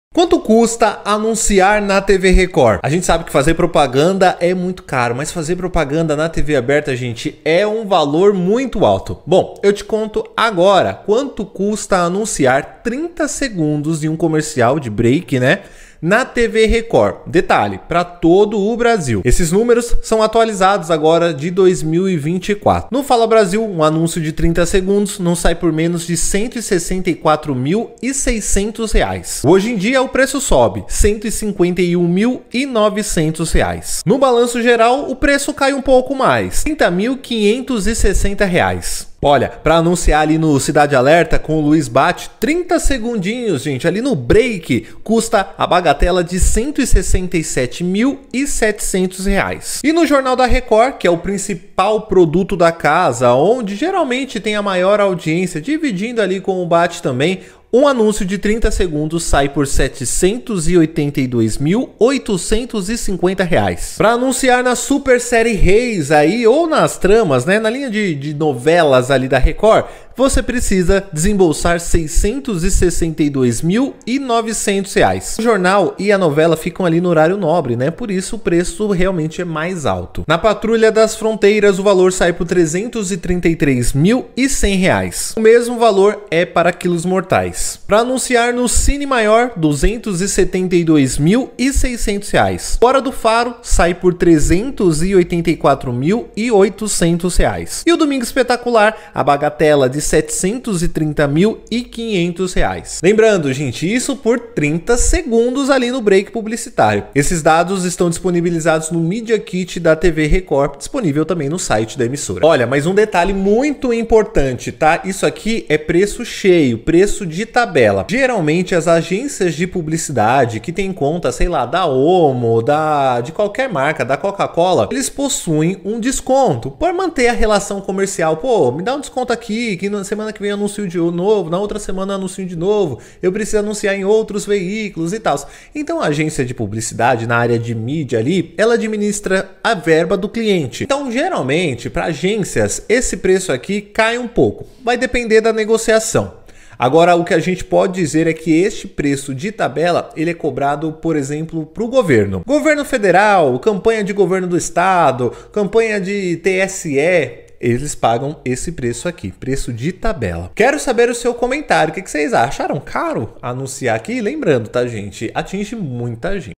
🚨 Quanto custa anunciar na Record TV! Valores CHOCANTES são divulgados por 30 segundos
Valor para Anunciar nos Programas da Record TV:
Referência: 2024
Duração: 30 segundos
Abrangência: Todo o Brasil

Balanço Geral Manhã: R$ 17.070,00
Fala Brasil: R$ 164.600,00
Hoje em Dia: R$ 151.900,00
Balanço Geral Tarde: R$ 30.560,00
Chicado PD: R$ 262.500,00
A Terra Prometida: R$ 191.300,00
Cidade Alerta: R$ 167.700,00
Jornal da Record: R$ 782.850,00
Série Reis: R$ 662.900,00
Gênesis: R$ 521.600,00
Futebol (quarta-feira): R$ 701.700,00
Futebol (domingo): R$ 701.700,00
Doc Investigação: R$ 333.100,00
Cine Record: R$ 333.100,00
Patrulha das Fronteiras: R$ 333.100,00
Câmera Record (quinta-feira): R$ 333.100,00
Câmera Record (domingo): R$ 355.200,00
Quilos Mortais: R$ 333.100,00
Brasil Caminhoneiro: R$ 134.700,00
Cine Aventura: R$ 113.900,00
Super Tela: R$ 333.100,00
Record Kids: R$ 19.000,00
Cine Maior: R$ 272.600,00
Hora do Faro: R$ 384.800,00
Domingo Espetacular: R$ 730.500,00

Aqui a gente comenta sobre tudo que rola na televisão. O mais legal é ter a sua participação.

Comente também sobre o que você acha. A sua opinião é muito importante para conseguirmos fazer aquela discussão.